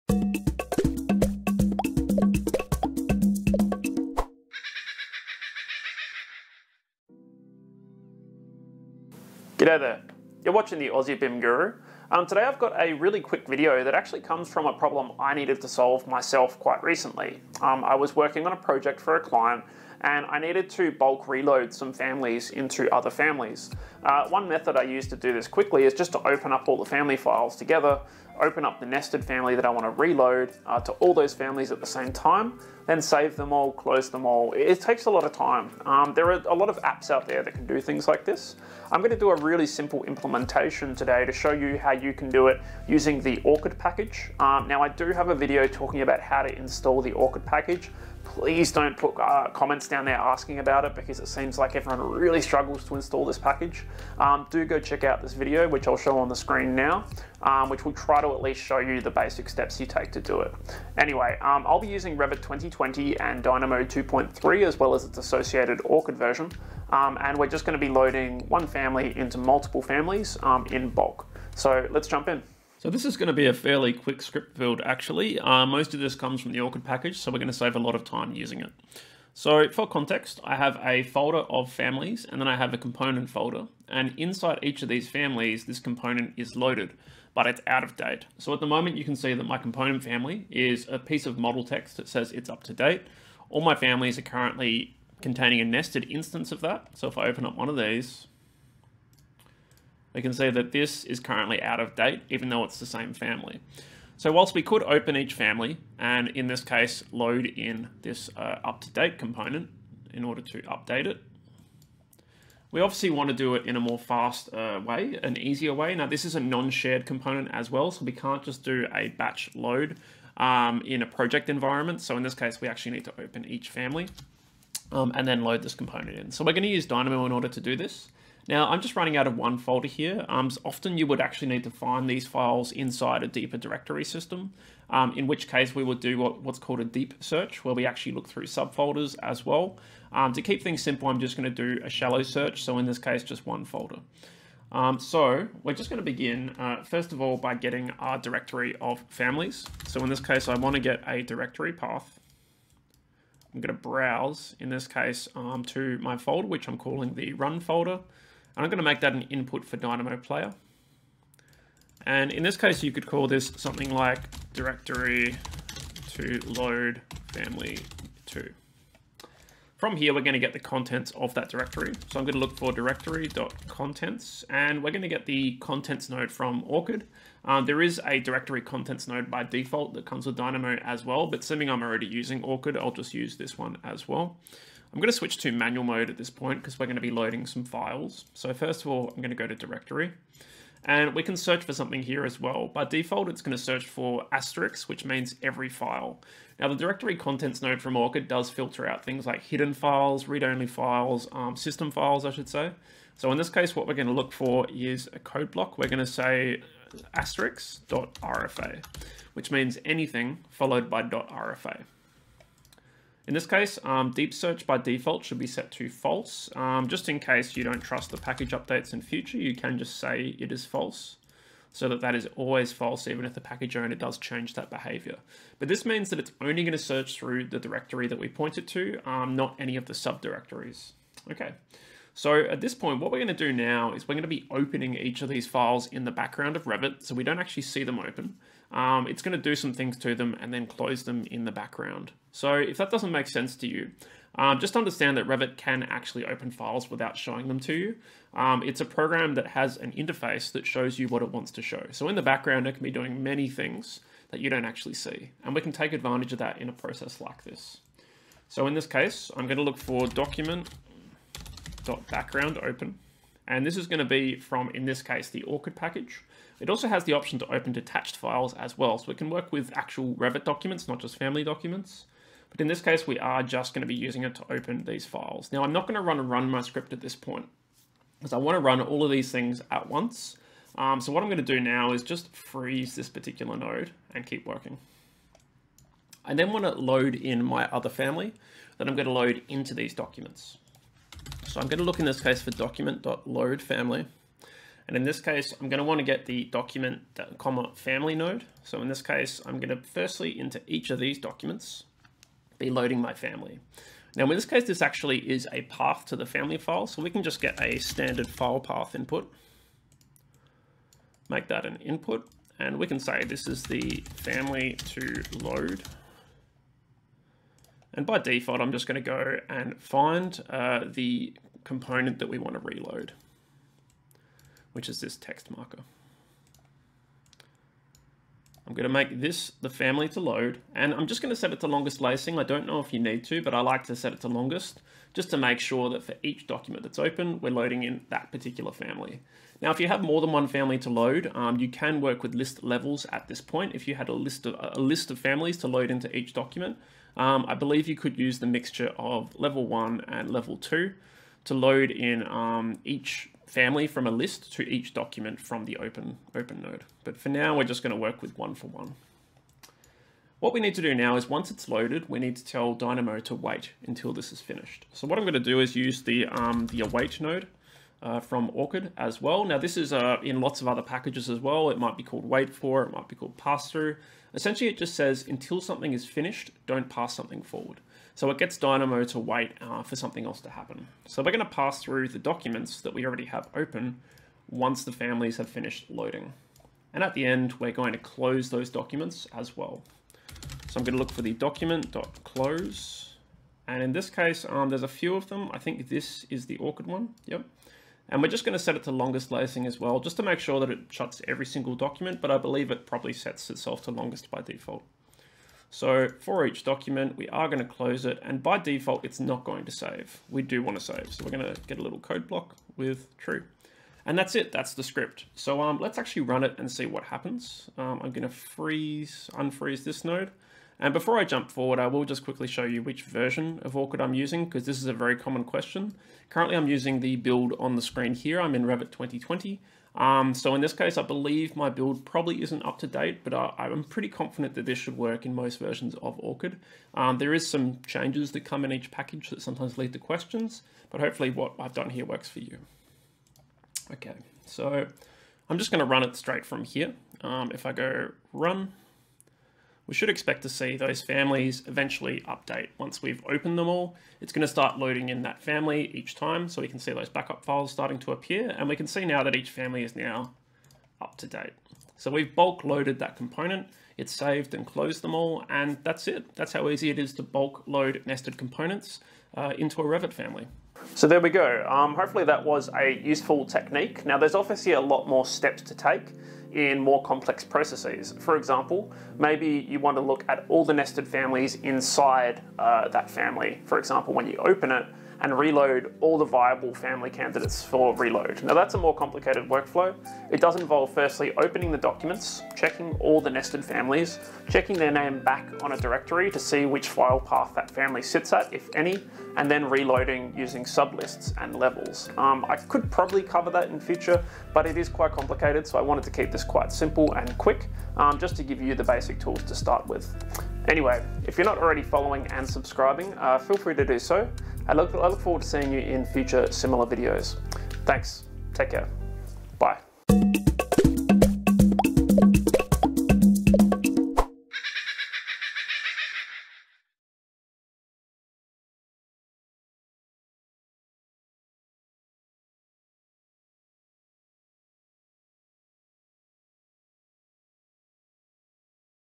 Bulk reload nested Revit families using Dynamo!
Handy script I developed to solve a real life problem I had the other day...

0:00 Intro
1:12 Family files
2:24 Scripting begins
7:00 Run the script
7:50 Outro

I hope you enjoy the session and learn something new!